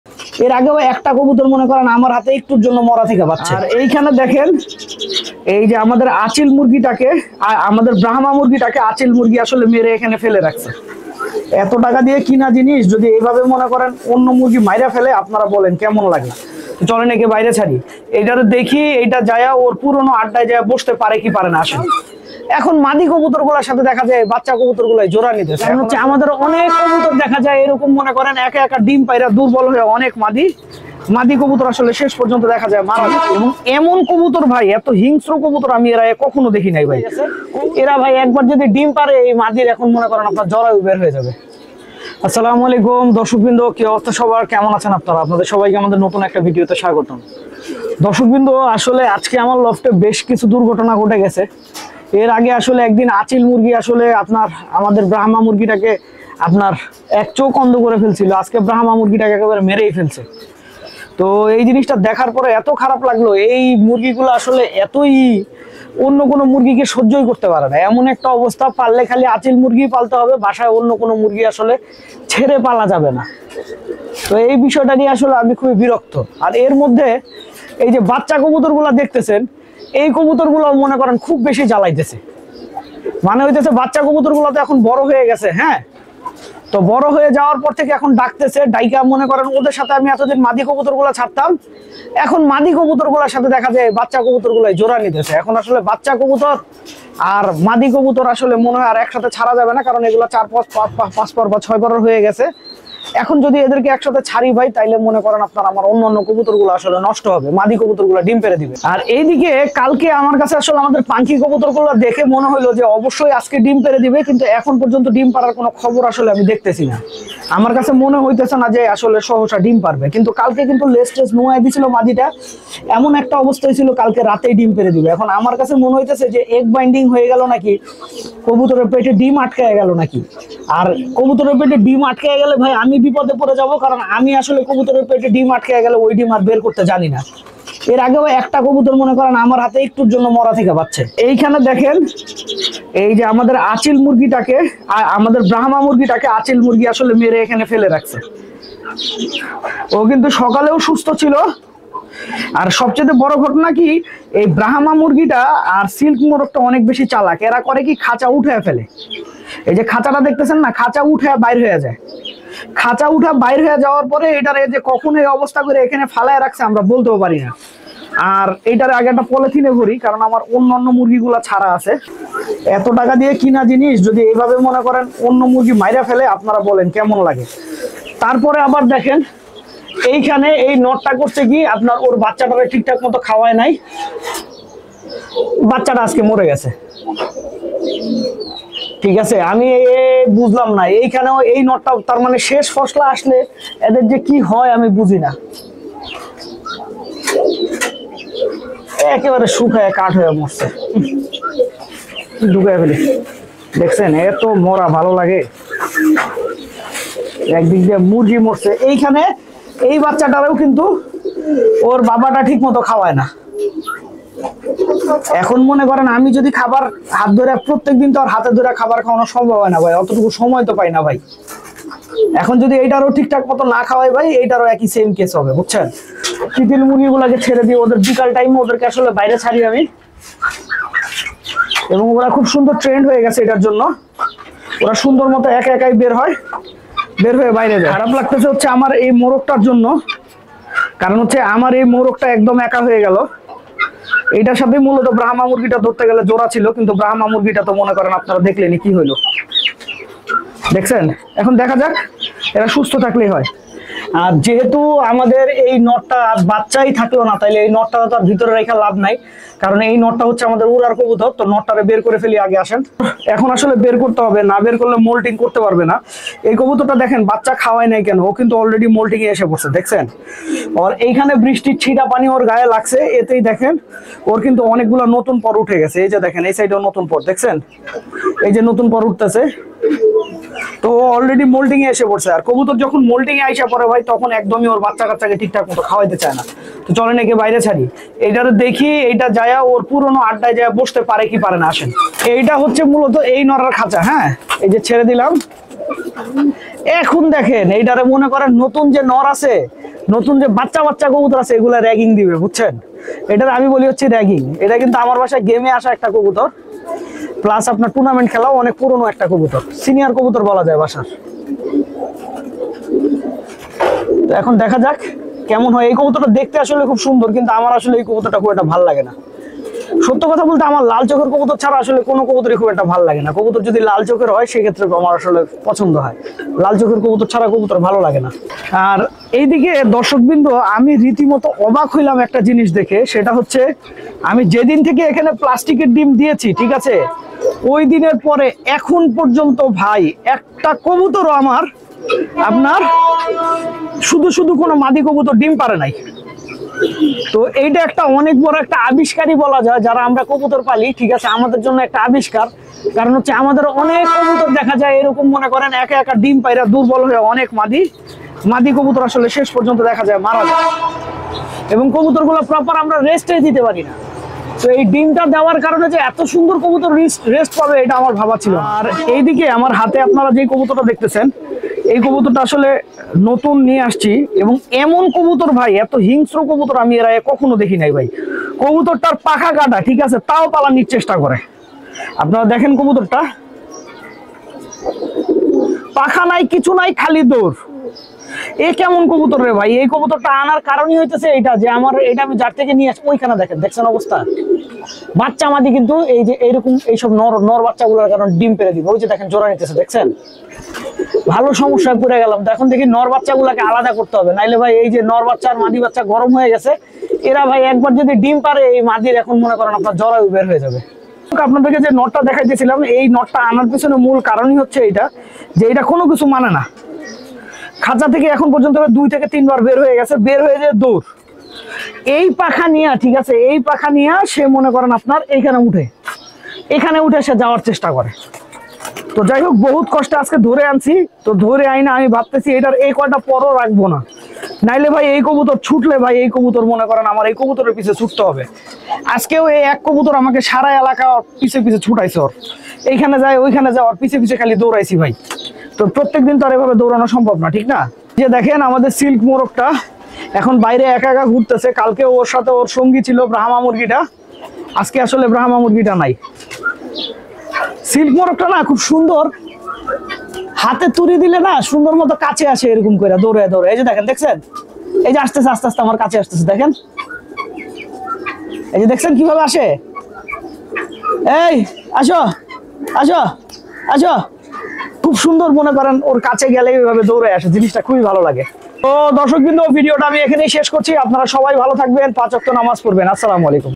আমার অনেক বড় ক্ষতি হয়ে গেল
আজকে আমাদের আঁচিল মুরগি গুলা ব্রাহমা মুরগিটাকে মেরে ফেলেছে সত্যিই জিনিসটা দেখার পর থেকে আমার মন খুব খারাপ